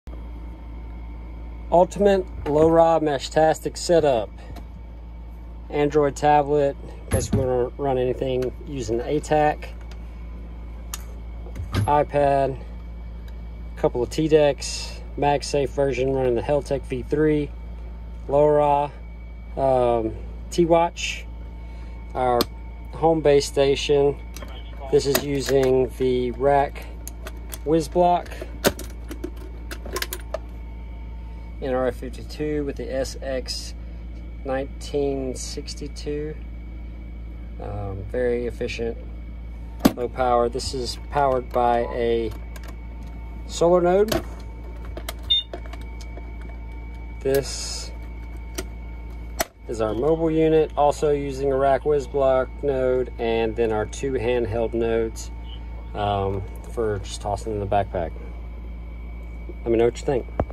Complete Off Grid Comms Kit Meshtastic + ATAK
The complete off-grid comms kit for Meshtastic and ATAK! The kit includes:

2 TDeck nodes (GPS ready)

2 handheld nodes (GPS ready)

1 TWatch node

1 mobile unit (GPS and solar charging enabled with panel)

1 homebase router node (6ft 8dBi gain antenna, solar charging enabled with panel for high location mount)